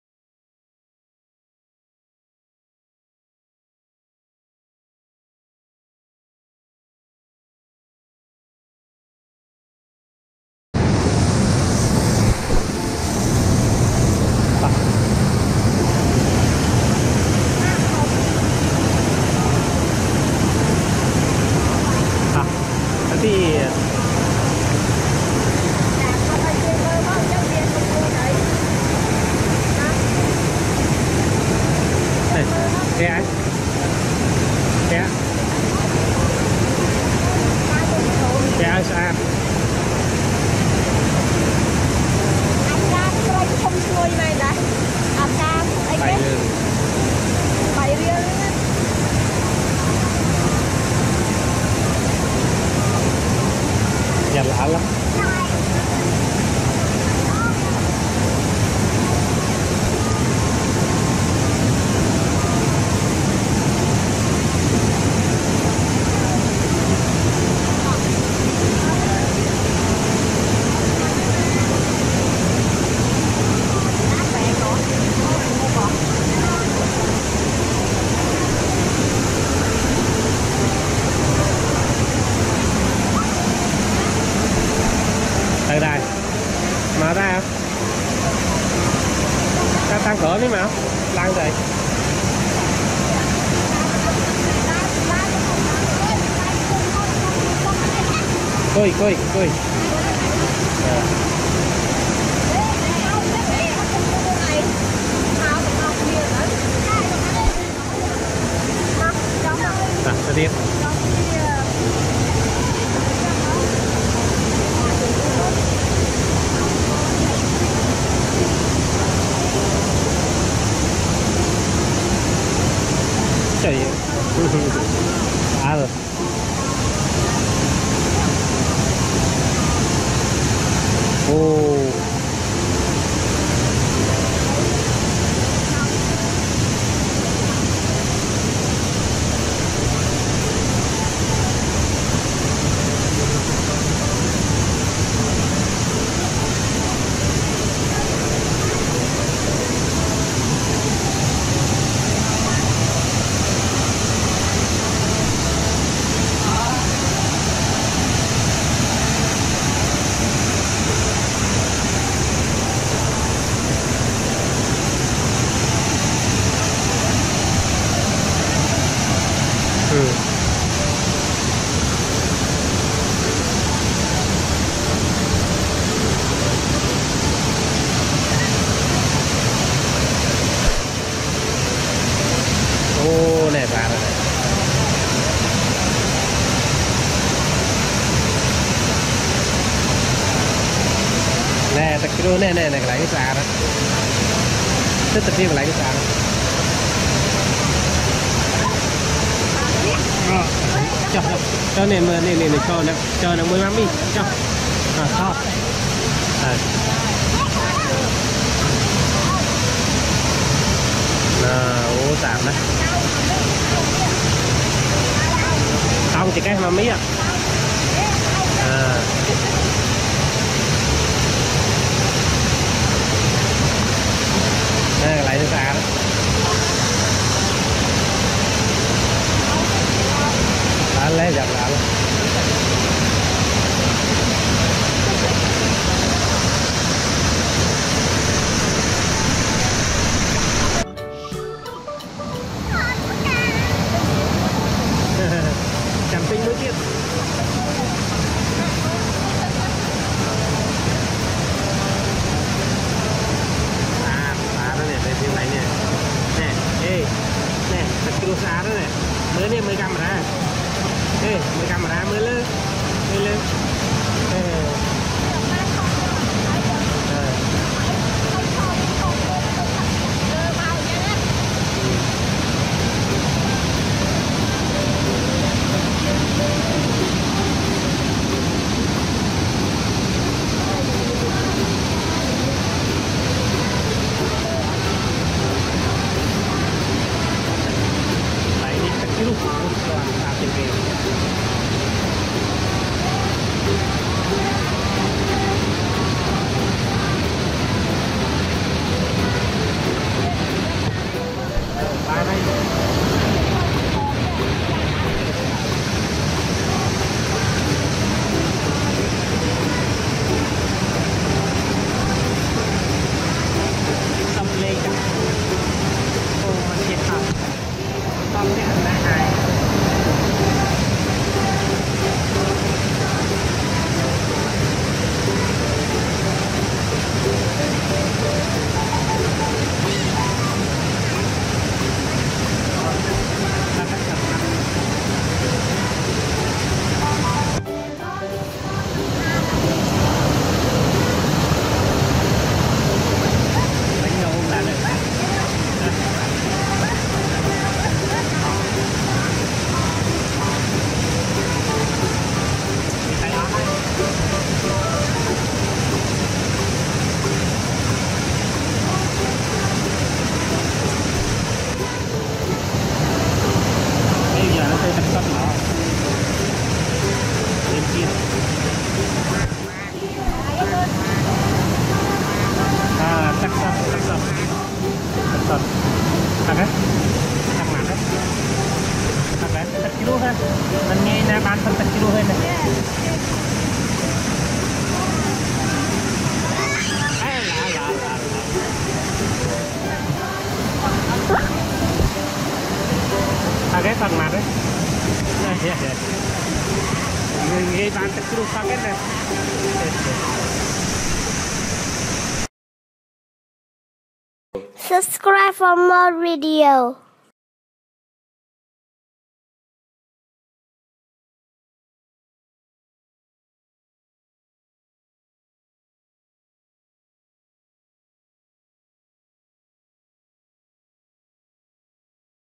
Jasmine Plays in the Waterfall 9 in Kampong Spue - Family Trip | Kids Channel
It is about kid videos. We will show you the kid Jasmine Plays in the Waterfall 9 in Kampong Spue - Family Trip | Kids Channel.